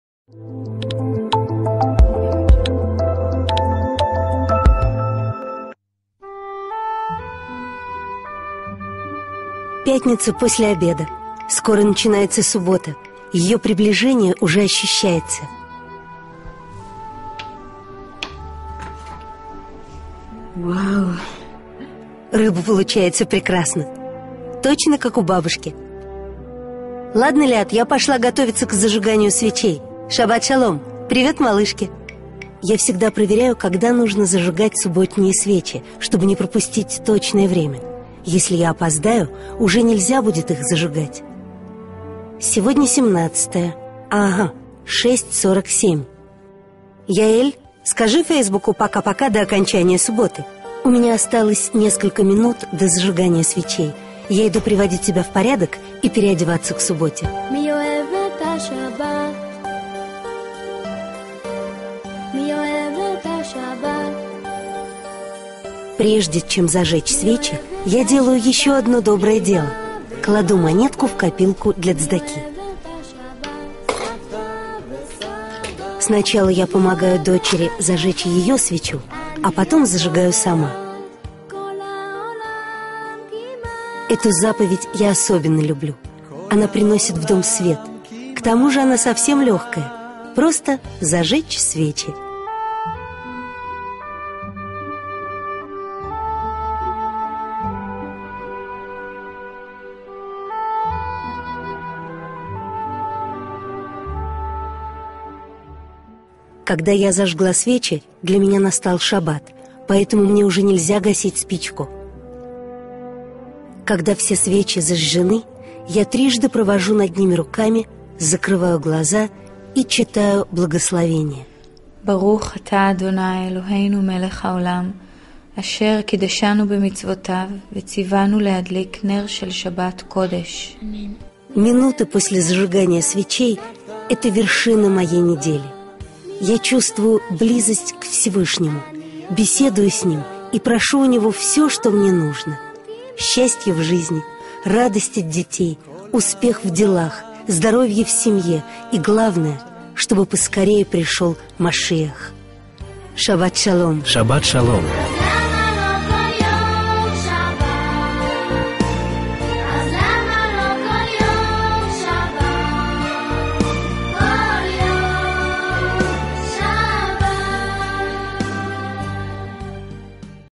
Tradições Judaicas: Nerot shabat
Um pequeno vídeo com as tradições de nerot shabat.

Meus livros:
A DISPUTA DE BARCELONA

NAFTULEI ELOKIM

TRADIÇÃO E DEFESA DA FÉ NOS ESCRITOS DO RABI SAUL LEVI MORTERA

PÉROLAS DO BAAL SHEM TOV